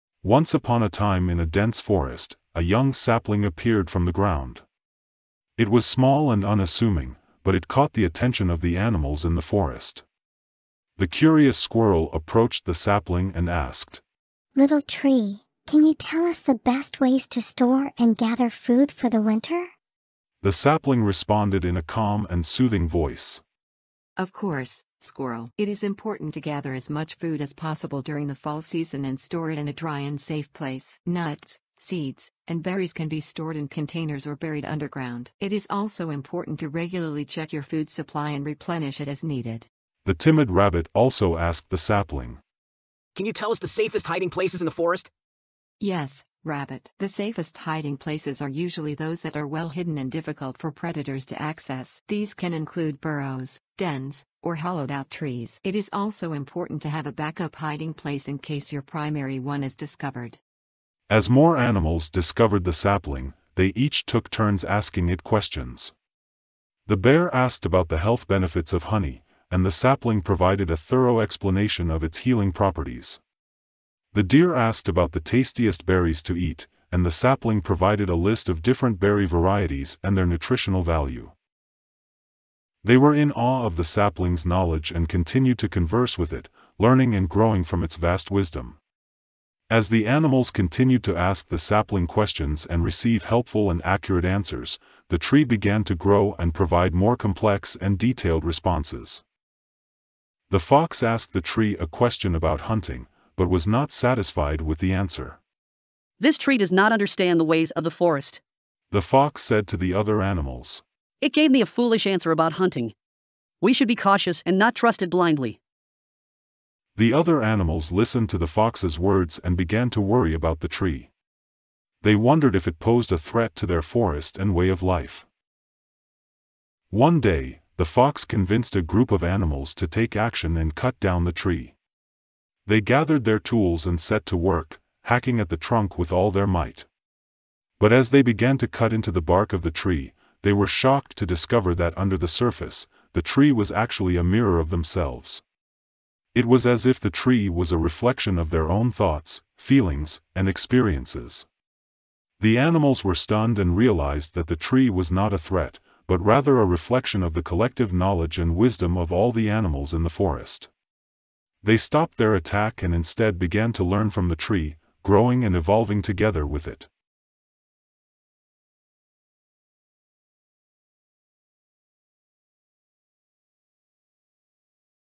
AI makes a story where animals discover ChatGPT - AI Fable
This is AI generated story, which was refined and tweaked overtime (maybe around 50 prompts) from few basic ideas and prompts given Chat GPT.